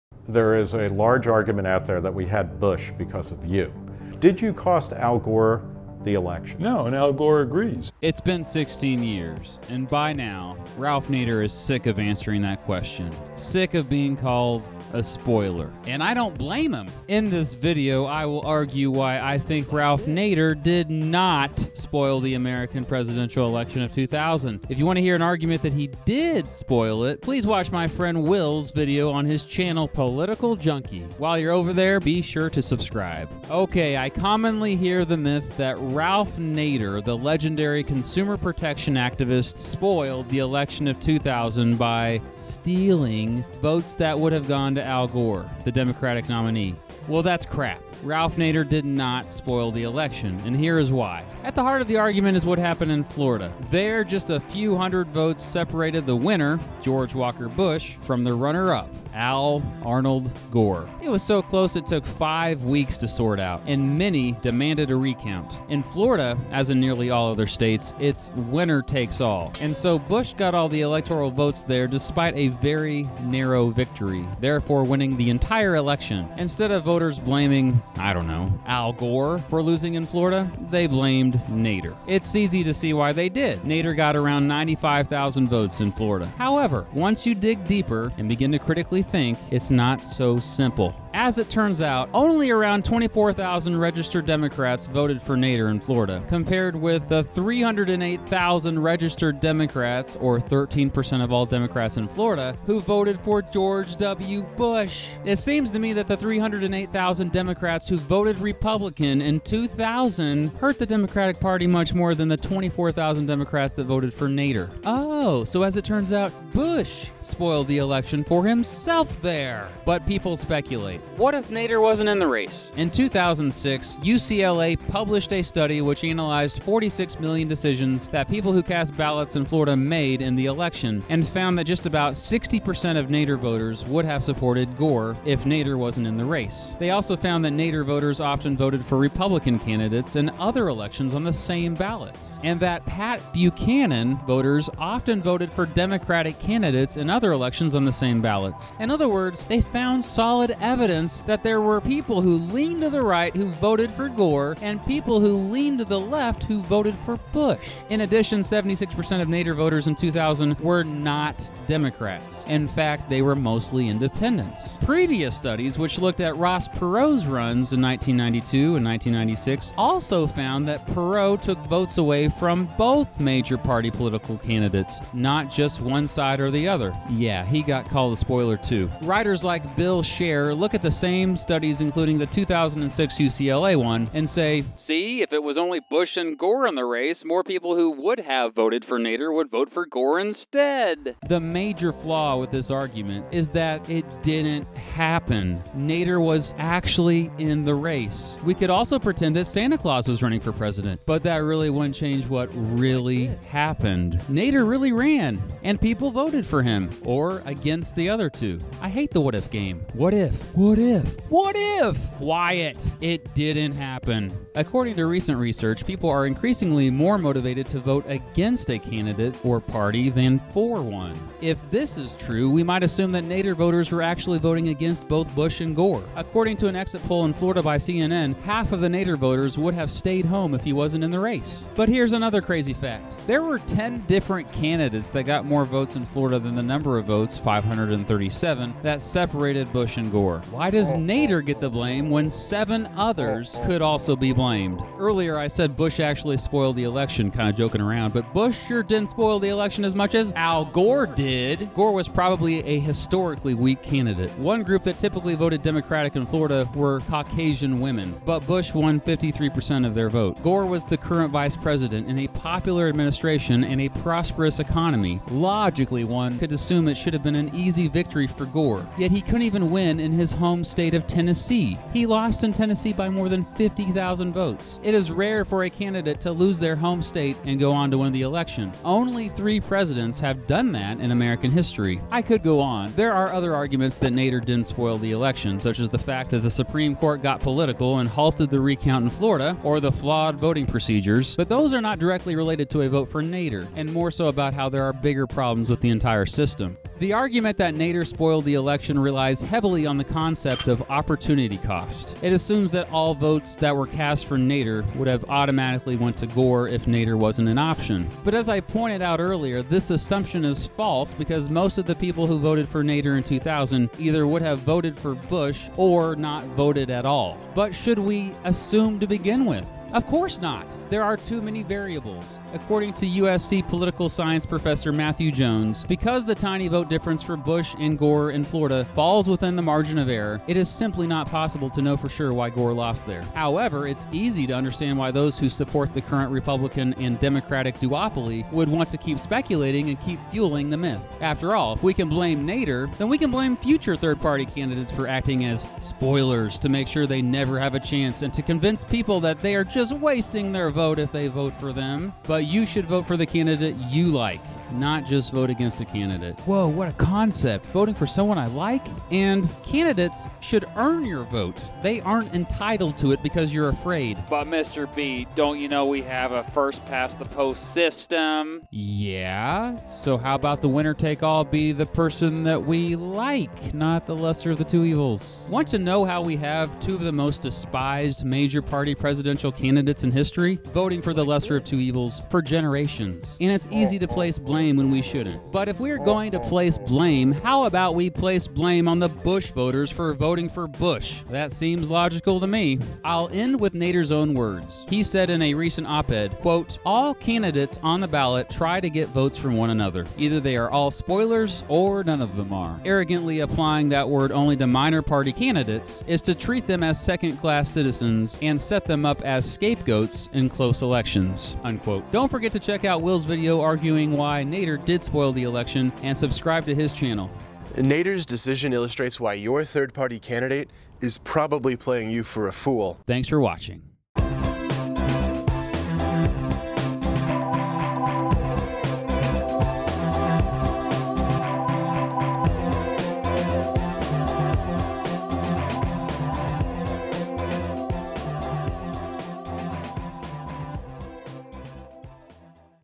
No, Ralph Nader Did Not Spoil the 2000 Election
Did Nader play the role of spoiler? In my first video collaboration with my friend Will from Political Junkie, I provide evidence why Ralph Nader absolutely did not spoil the presidential election of 2000. If anyone did, it's probably George W. Bush.

Ok, I commonly hear the myth that Ralph Nader, the legendary consumer protection activist, spoiled the election of 2000 by “stealing” votes that would have gone to Al Gore, the Democratic nominee. Well, that’s crap. Ralph Nader did not spoil the election, and here is why.

At the heart of the argument is what happened in Florida. There, just a few hundred votes separated the winner, George Walker Bush, from the runner up, Al Arnold Gore. It was so close it took five weeks to sort out, and many demanded a recount. In Florida, as in nearly all other states, it’s “winner takes all,” and so Bush got all the electoral votes there despite a very narrow victory, therefore winning the entire election. Instead of voters blaming, I don’t know, Al Gore, for losing in Florida, they blamed Nader.

It’s easy to see why they did. Nader got around 95,000 votes in Florida.

However, once you dig deeper and begin to critically think, it’s not so simple. As it turns out, only around 24,000 registered Democrats voted for Nader in Florida, compared with the 308,000 registered Democrats (or 13 percent of all Democrats in Florida) who voted for George W. Bush. It seems to me that the 308,000 Democrats who voted Republican in 2000 hurt the Democratic Party much more than the 24,000 Democrats that voted for Nader. Oh! So, as it turns out, Bush spoiled the election for himself there.

But people speculate. What if Nader wasn’t in the race? In 2006, UCLA published a study which analyzed 46 million decisions that people who cast ballots in Florida made in the election, and found that just about 60% of Nader voters would have supported Gore if Nader wasn’t in the race. They also found that Nader voters often voted for Republican candidates in other elections on the same ballots, and that Pat Buchanan voters often voted for Democratic candidates in other elections on the same ballots. In other words, they found solid evidence that there were people who leaned to the right who voted for Gore and people who leaned to the left who voted for Bush. In addition, 76% of Nader voters in 2000 were not Democrats- in fact, they were mostly independents.

Previous studies, which looked at Ross Perot’s runs in 1992 and 1996 also found that Perot took votes away from both major party political candidates, not just one side or the other.

Writers like Bill Scher look at the same studies, including the 2006 UCLA one, and say “see, if it was only Bush and Gore in the race, more people who would have voted for Nader would vote for Gore instead!” The major flaw with this argument is that that didn’t happen. Nader was actually in the race. We could also pretend that Santa Claus was running for President, but that really wouldn’t change what really happened. Nader really ran, and people voted for him, or against the other two. I hate the “what if” game. What if, what if, what if- QUIET, IT DIDN’T HAPPEN. According to recent research, people are increasingly more motivated to vote against a candidate or party than for one. If this is true, we might assume that Nader voters were actually voting against both Bush and Gore. According to an exit poll in Florida by CNN, half of Nader voters would have stayed home if he wasn’t in the race.

But here’s another crazy fact- there were ten different candidates that got more votes in Florida the the number of votes, 537, that separated Bush and Gore. Why does Nader get the blame when seven others could also be blamed?